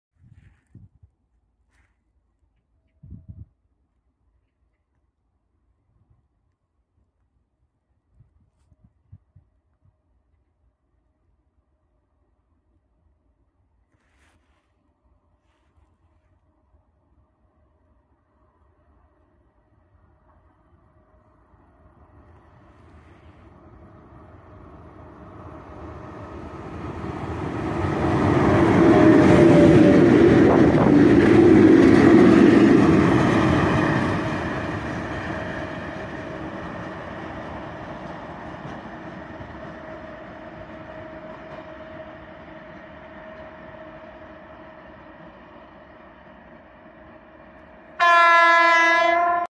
Апрельский одиночка. 3ЭС5К-321 "Ермак" с приветливой бригадой следует резервом
Местный электровоз 3ЭС5К-321 "Ермак" с приветливой бригадой следует резервом по перегону Амурский Залив - Надеждинкая пасмурным весенним днём.(13:34)
Дорога приписки:Дальневосточная ж/д.
Депо приписки:ТЧЭ-2 Хабаровск-II.
Построен:08.2012 г.
Сетевой №:11381068/11381076/11381084.
Дата съёмки:3 апреля 2021 года.